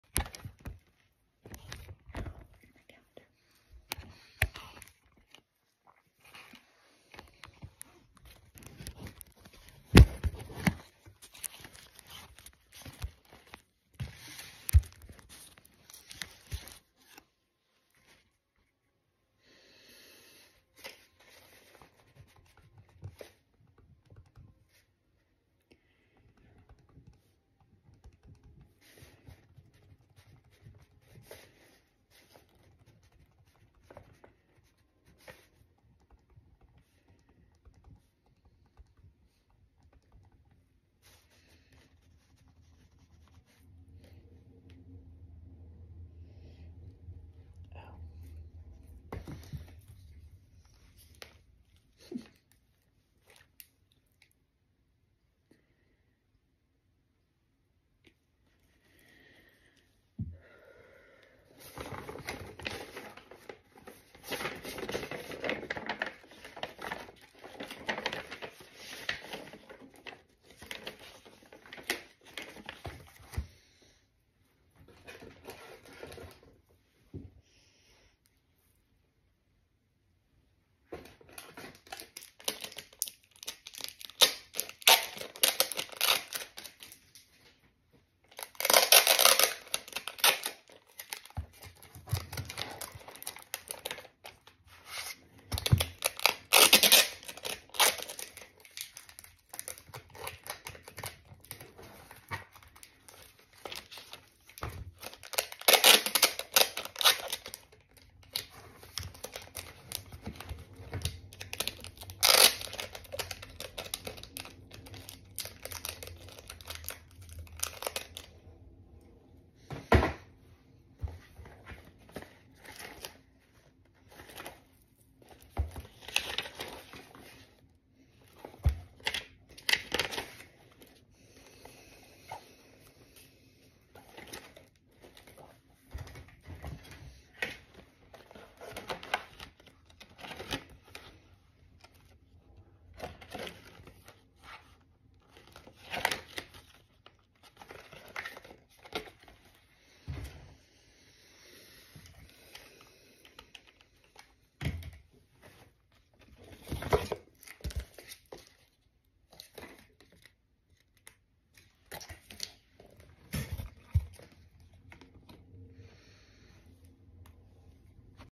Be a Girl boss !!!!
Noise cancellation headphones , a gift I bought for me !!! Game changer planning each week I write my subscribers, I need 1,000 I am at 446 subscribers 163,339 views, 2.6k videos goals ! Work your plan , plan your work oldfashion oldschool writeaplan listen to prayers on hallow